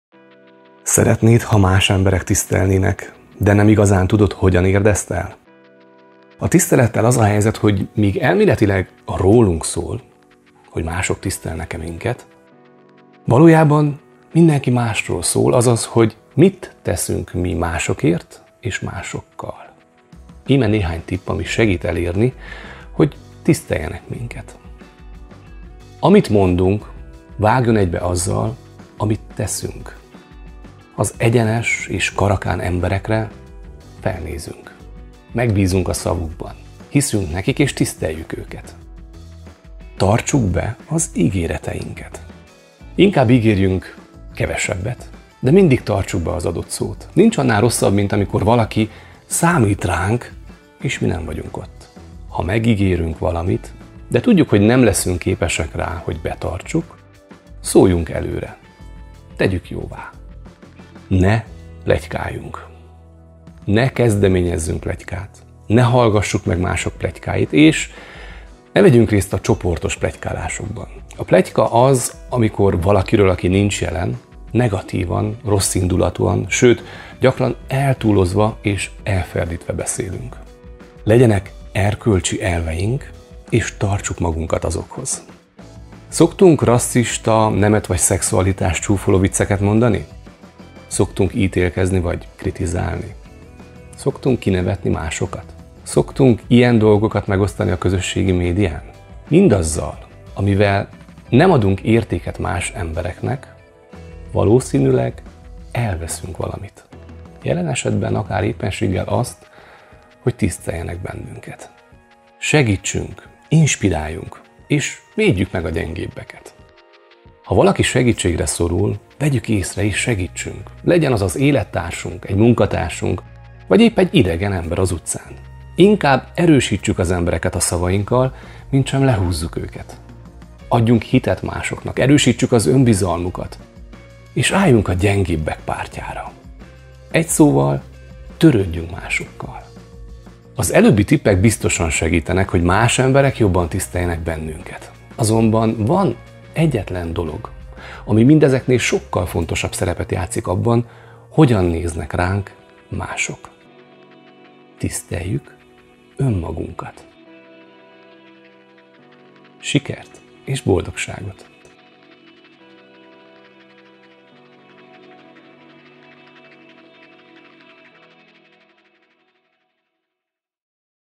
5+1 tipp, hogy tiszteljenek az emberek
Szeretnéd, ha más emberek tisztelnének, de nem igazán tudod, hogyan érd ezt el? A tisztelettel az a helyzet, hogy míg elméletileg rólunk szól - hogy mások tisztelnek-e minket -, valójában mindenki másról szól, azaz hogy mit teszünk mi másokért és másokkal. Íme néhány tipp, ami segít elérni, hogy tiszteljenek minket.

Sikert és boldogságot!